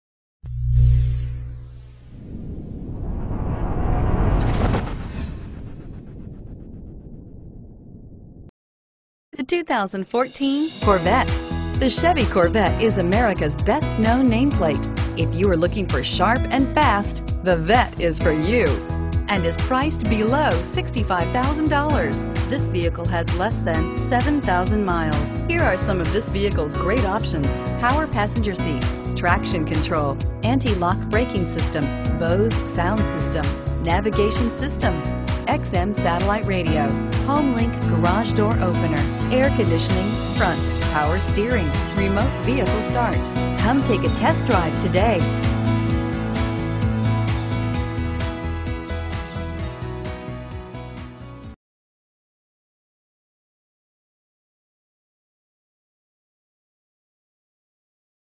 2014 Chevrolet Corvette Stingray Austin MN 31705
2014 Chevrolet Corvette Stingray Z51 3LT

Usem Inc.

Don't miss this 2014 Chevrolet Corvette Stingray. It's equipped with Automatic transmission and features a Arctic White exterior. With 6522 miles, You'll want to take this car home. Make a great choice today.